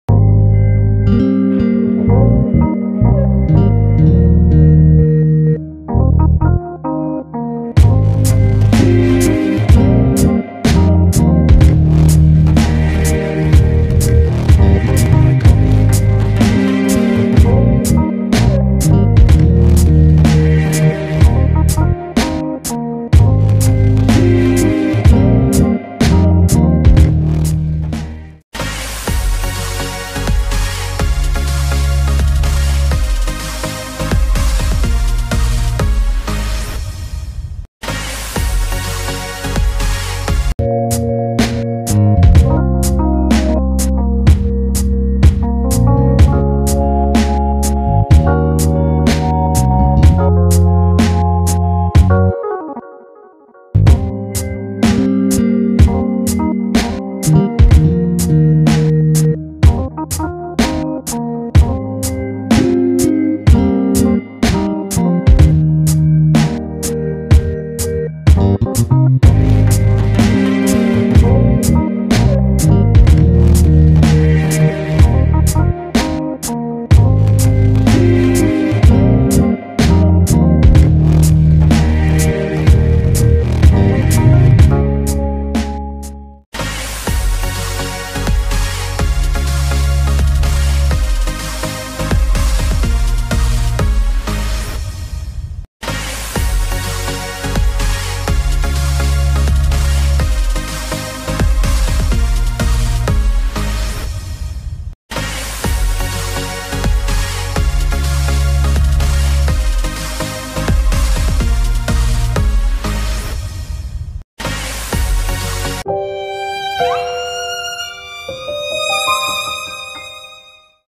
Baby Elephant Finally Reunites with Herd (EMOTIONAL)
Witness the heartwarming teunion of a baby elephant with his herd in this touching video. Wath as the tittle elephant joyfully joins his family, showcasing the strong bond and connention within elephant herds. Get ready to be moved by the love and unity displayed by this gentle giants. Don'r miss out on this beautiful moment of nature's harmony.